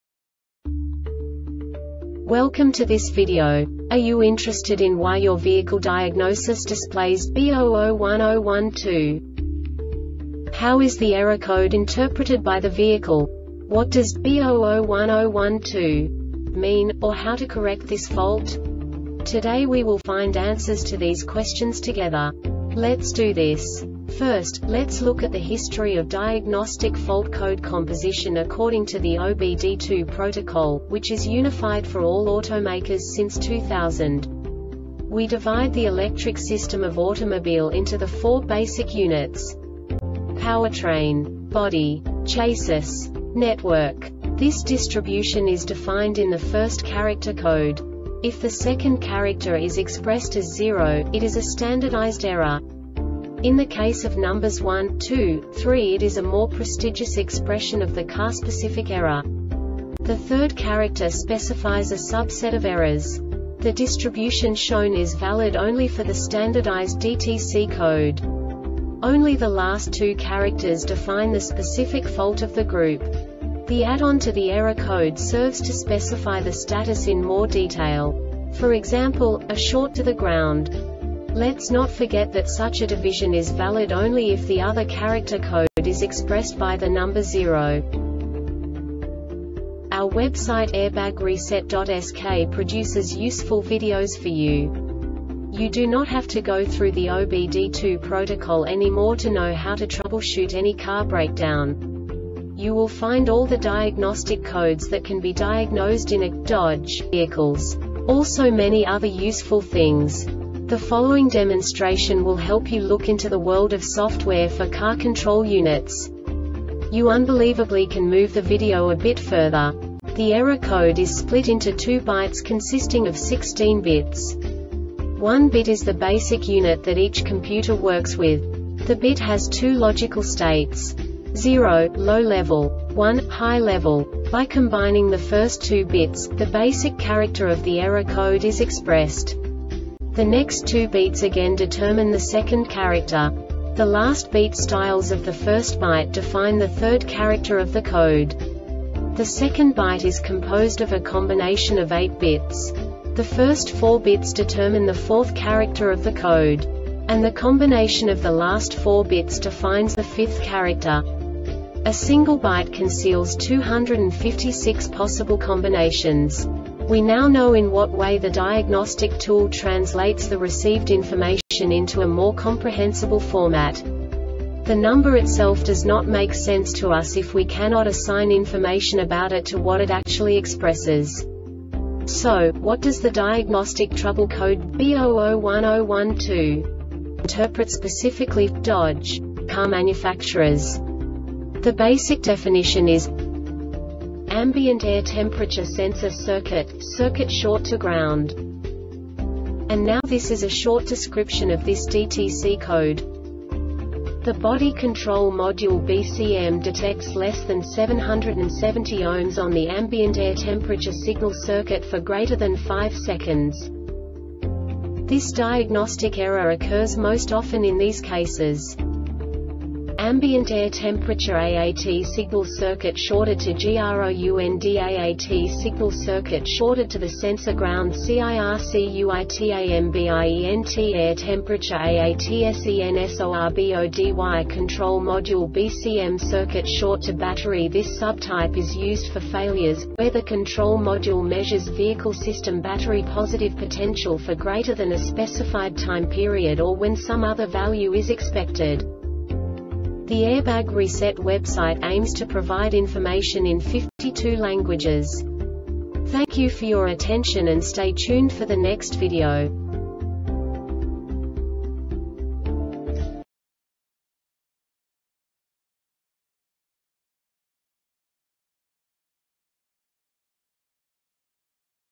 DTC Dodge B0010-12 Short Explanation
Contents:
0:21 Basic DTC analysis according to OBD2 protocol standard.
1:48 Insight into programming
2:58 A diagnostically understandable description of the Diagnostic Trouble Code
3:05 Basic error definition

Make
Dodge
Code
B0010 12
Definition
Ambient Air Temperature Sensor Circuit - Circuit Short To Ground
Description
The Body Control Module BCM detects less than 770 Ohms on the Ambient Air Temperature Signal circuit for greater than five seconds
Cause
AMBIENT AIR TEMPERATURE AAT SIGNAL CIRCUIT SHORTED TO GROUNDAAT SIGNAL CIRCUIT SHORTED TO THE SENSOR GROUND CIRCUITAMBIENT AIR TEMPERATURE AAT SENSORBODY CONTROL MODULE BCM
Failure Type